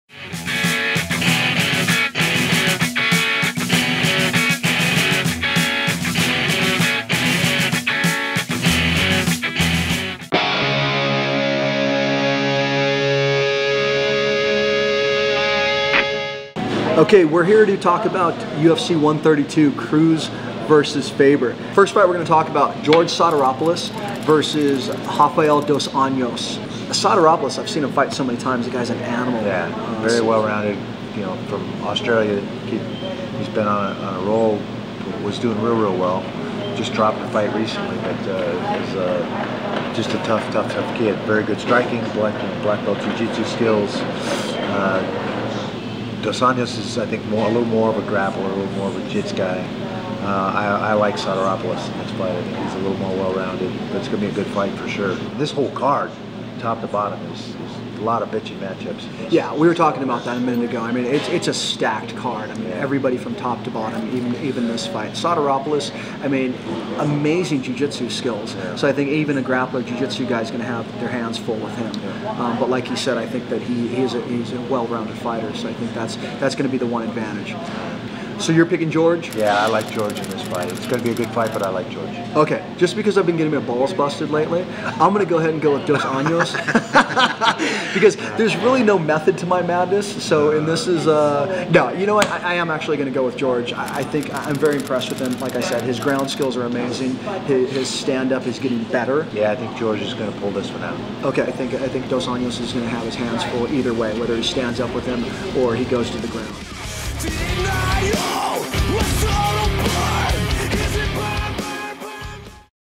Affliction's Randy Couture predicts UFC 132 Sotiropoulos vs Dos Anjos
Affliction's Tom Atencio and Randy Couture predict the winner of UFC 132 for the George Sotiropoulus vs Rafael Dos Anjos bout.